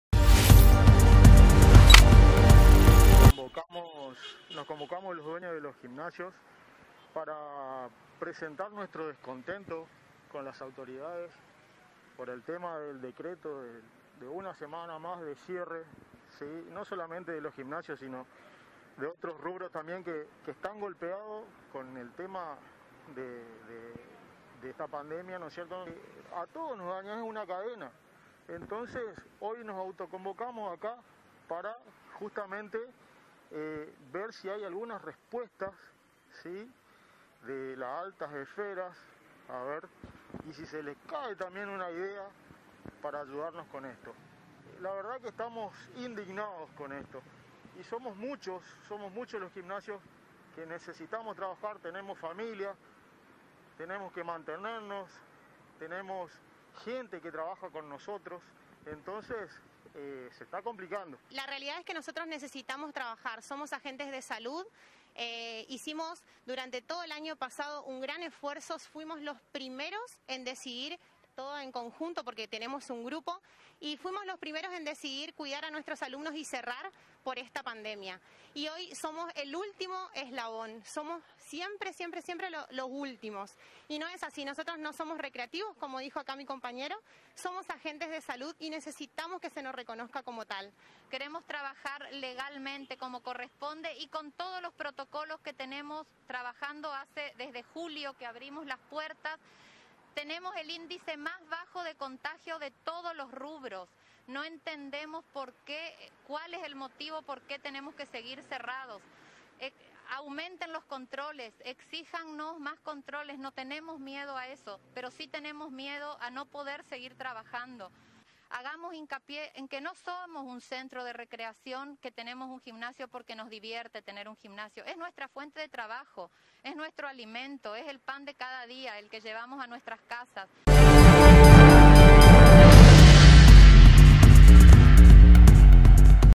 N9 FEDERAL - RESISTENCIA - GIMNASIOS PIDEN VOLVER A SUS ACTIVIDADES
SE CONCENTRARON PARA PROTESTAR Y PEDIR LA APERTURA DE SUS LOCALES
Los propietarios de gimnasios solicitan volver a sus actividades. Durante el año pasado sus locales permanecieron cerrados durante meses hasta que se logró establecer un protocolo para su atención, piden volver a esa modalidad.